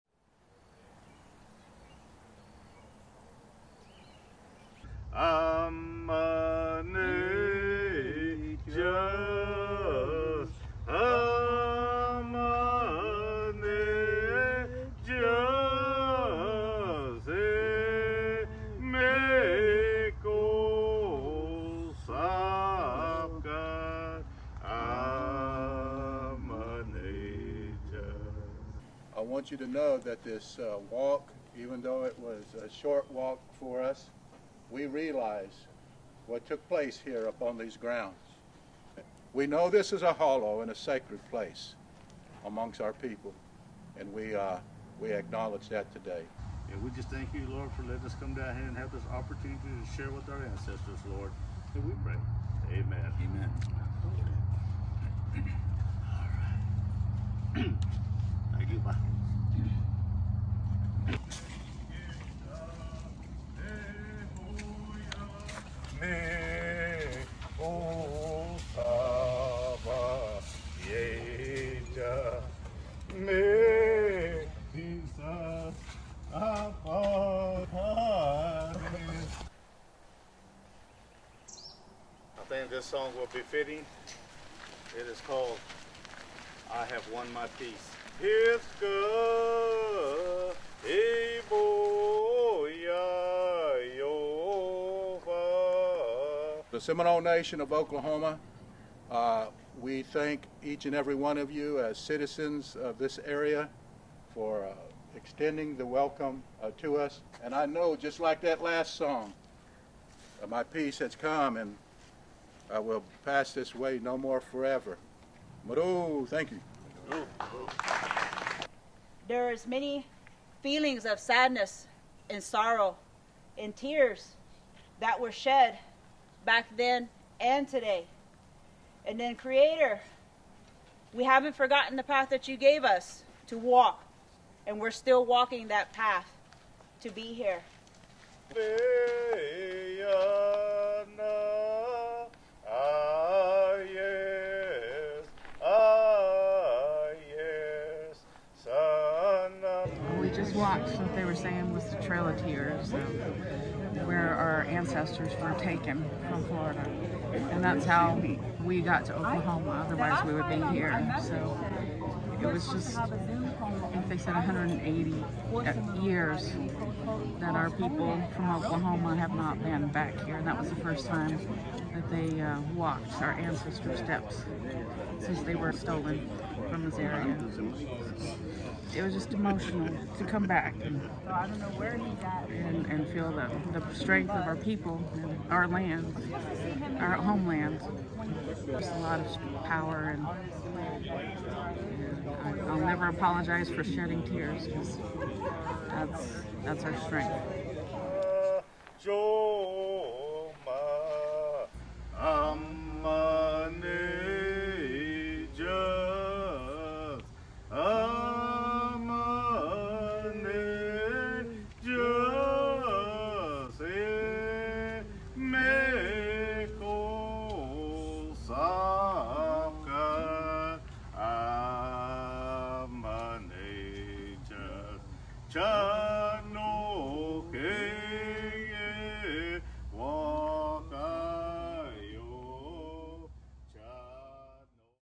Oklahoma Seminoles retrace their Florida roots
Full story at ByJoeCapozzi.com